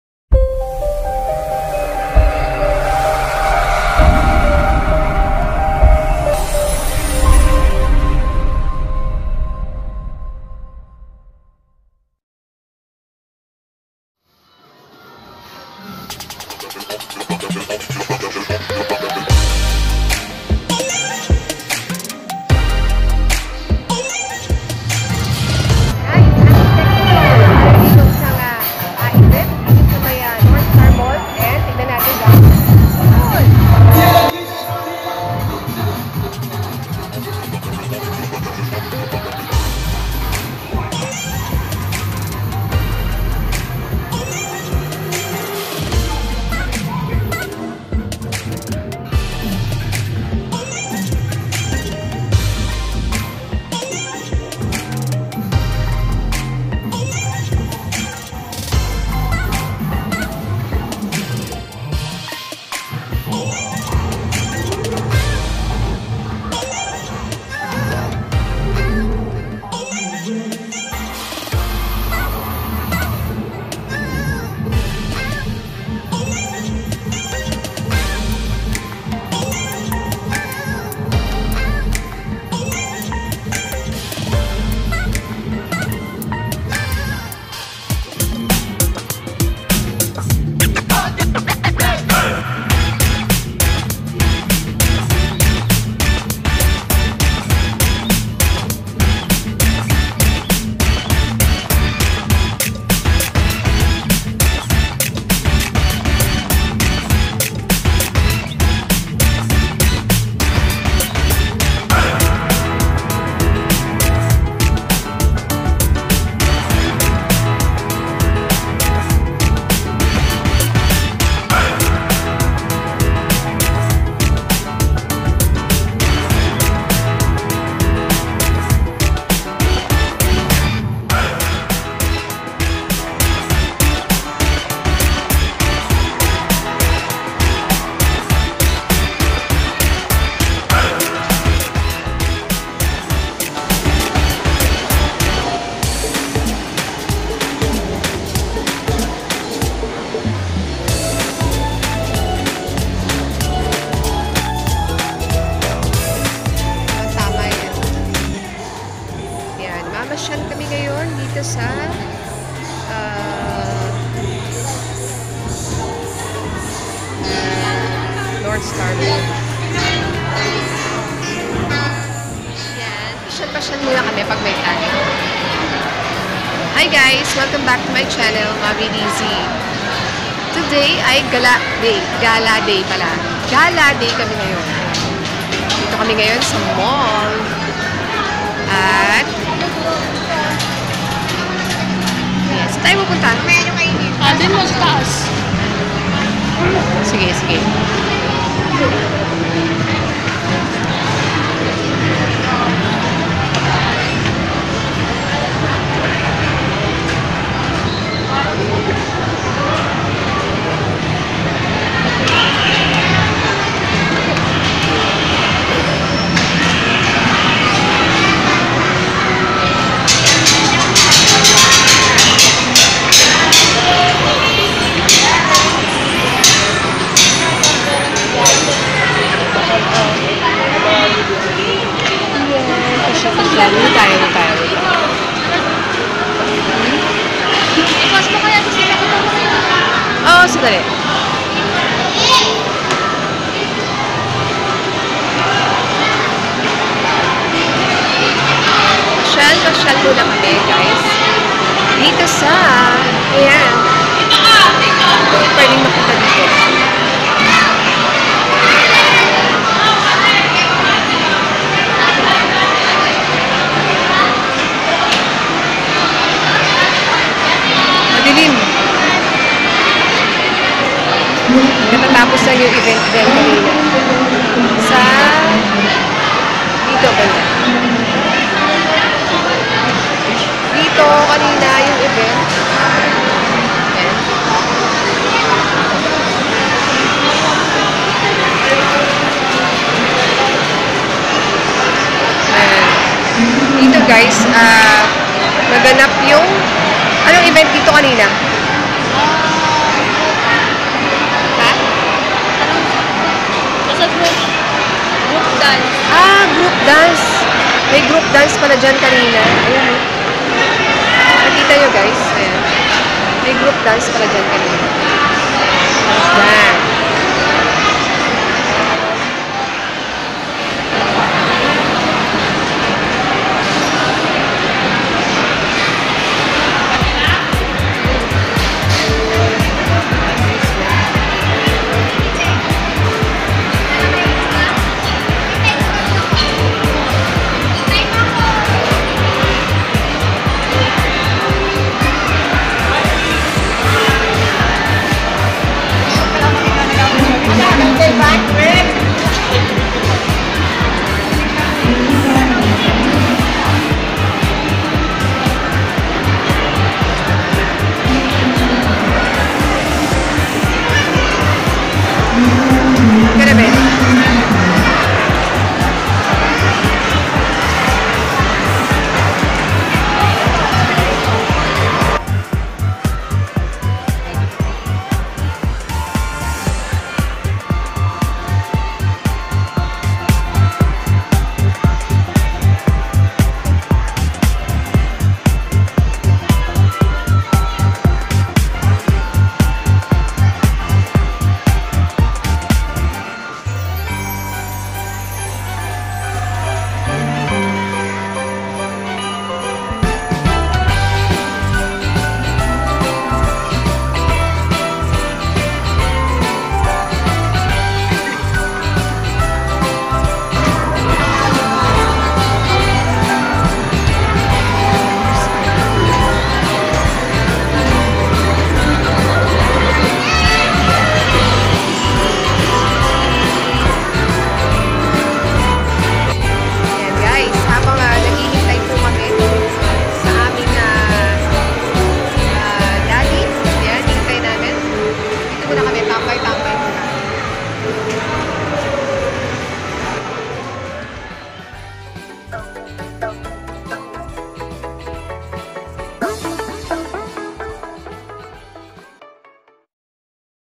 One Way To Feel Good - Malling With Family
Hi guys! Today is gala day because i just wanna make myself feel good and take off all my frustrations and sadness kaya panoorin nyo kung paano nga ba mabawasan ang bad vibes while waiting for my hubby..so please watch till the end and subscribe to my channel and don't forget to hit the notification bell para sa mga susunod ko pang uploads. Thanks!